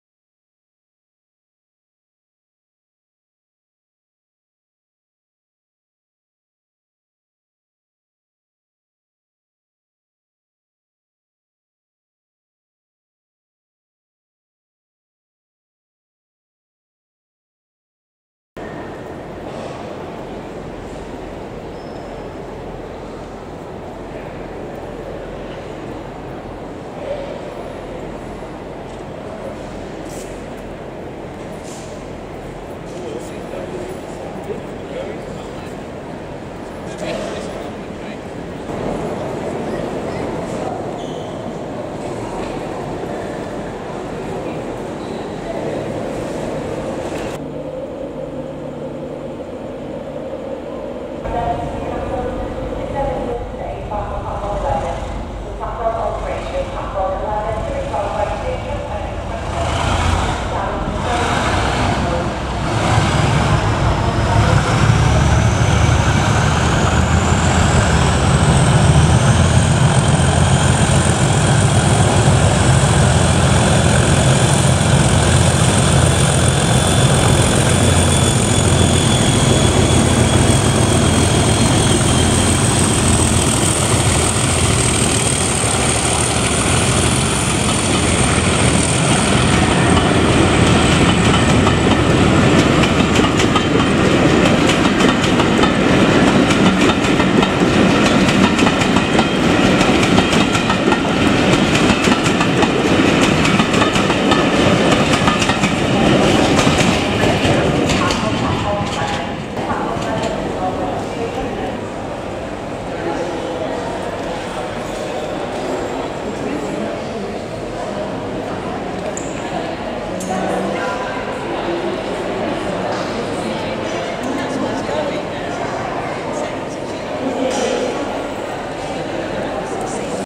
DRS Class 37s Nos 37059 & 37038 at York - 'The Yorkshire Explorer' Rail Tour - 8th June 2019
'The Yorkshire Explorer' rail tour from Taunton to Leeds thrashes out of York with DRS Class 37 locos Nos. 37059 and 37038 double heading the train.